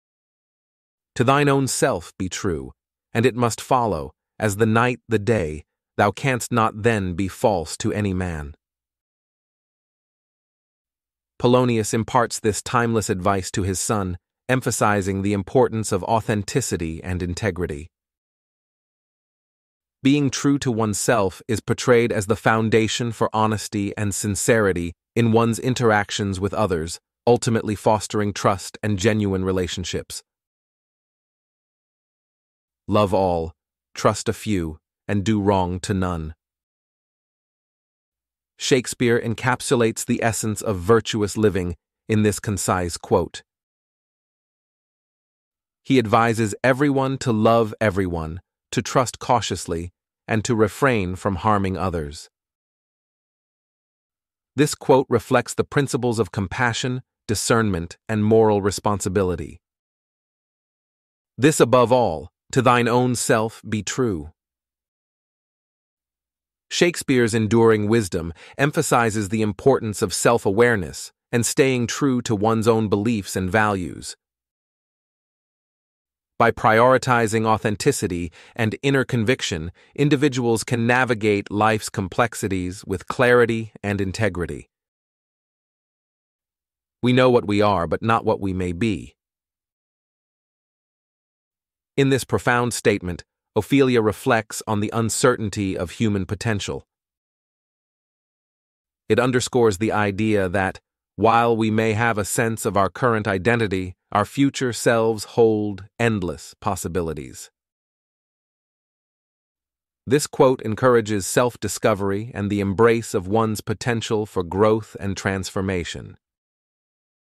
William Shakespearean Wisdom: Insights for Life's Journey @Mr.NonstopWisdom
Delve into the timeless wisdom of William Shakespeare with this collection of profound quotes from his iconic plays. From reflections on the human condition to insights into love, trust, and self-discovery, Shakespeare's words continue to resonate across centuries, offering guidance and inspiration for navigating life's journey. Explore the depths of the Bard's wisdom and uncover invaluable lessons that remain as relevant today as they were in Shakespeare's time. Whether seeking solace in times of uncertainty or pondering life's complexities, "Shakespearean Wisdom" is a companion for anyone seeking deeper understanding and timeless